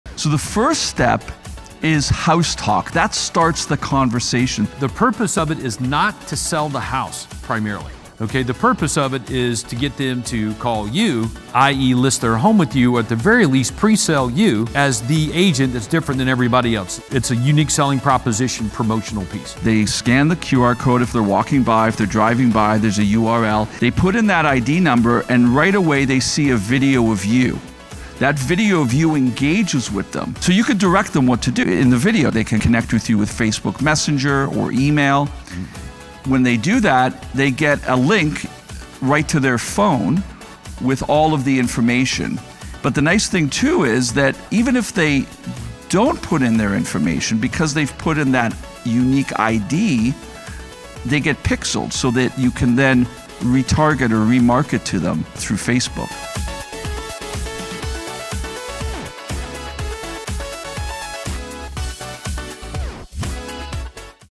Leverage Your Listings to Get MORE LISTINGS!
Add HouseTalk to Your Lead Generation System, a part of the Exclusive and Innovative Craig Proctor System.

YOUR SELLERS WILL LOVE HOUSETALK!

The secondary sign in front of the property attracts the attention of buyers walking or driving around looking at the neighborhood. Signage is a proven way to attract prospects to your seller’s property and HouseTalk “starts the conversation” with them.

With HouseTalk you can provide Instant Audio, Video, Photo and Text information on your listed properties right to your prospect's Smartphone, Tablet or Computer from signage, on social media or through print. You get the conversation started with prospects.